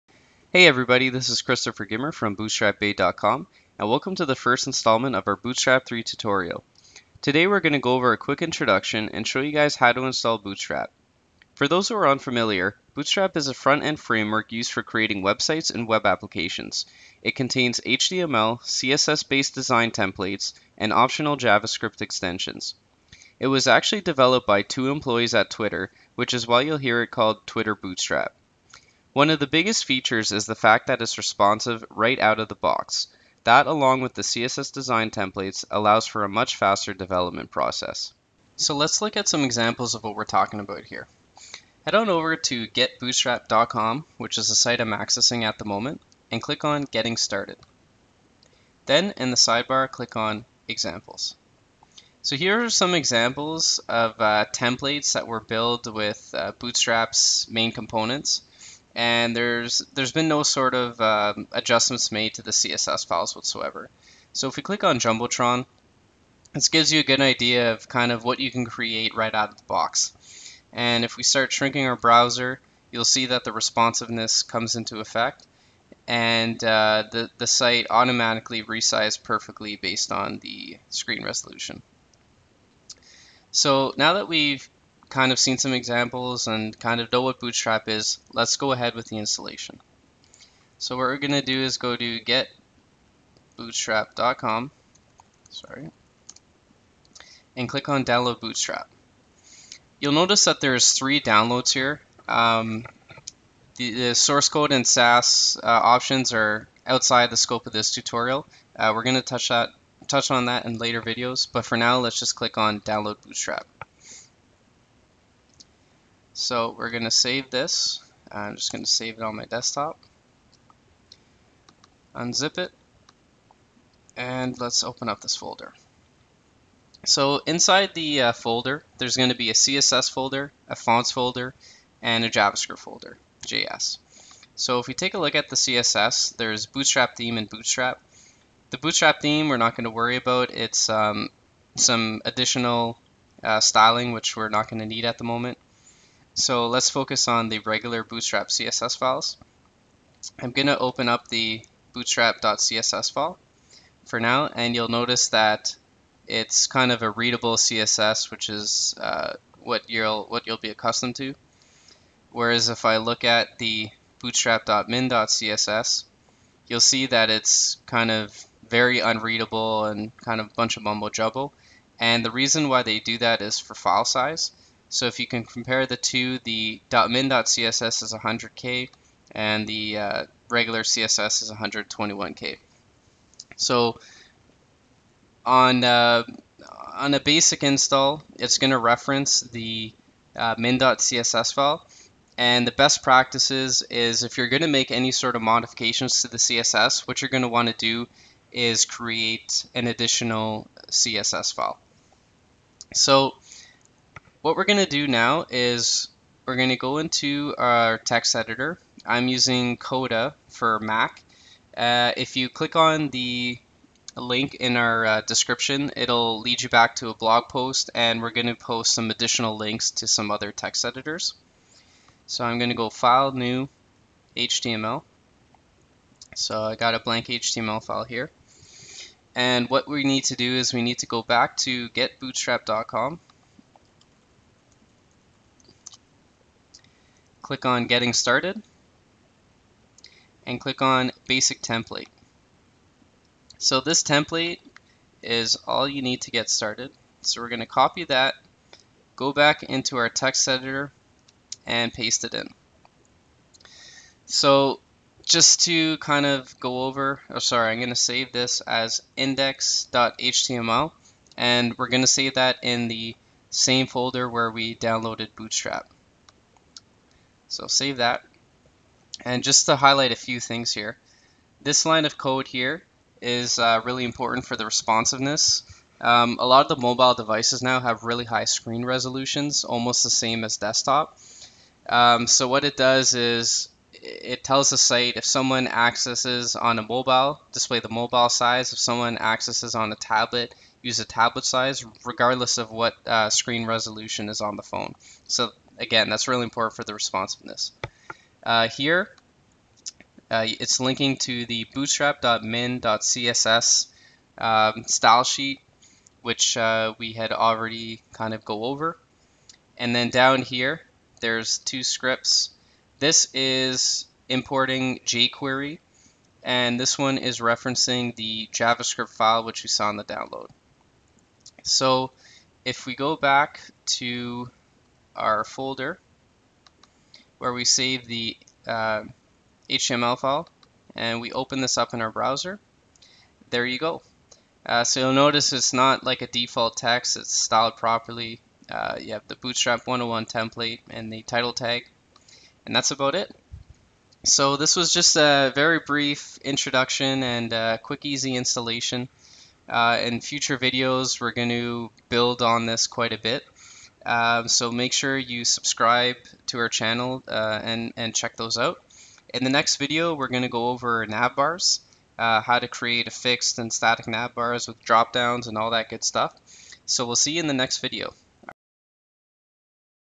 Bootstrap 3 Tutorial Pt.1 - Intro & How to Install Bootstrap 3
In this video, we give an introduction of Bootstrap and a detailed tutorial on how to install Bootstrap 3.

Bootstrap is the most popular front-end framework for developing responsive, mobile first projects on the web.

Video Highlights:

0:11 - Introduction of Bootstrap
0:41 - Accessing getBootstrap to look at some examples
0:52 - Examples of templates that are built with Bootstrap
1:30 - How to download Bootstrap from getBootstrap
2:07 - We go over the contents of the downloaded folder
3:23 - How to install the default Bootstrap template
4:28 - An overview of the HTML code
5:22 - Testing the Bootstrap installation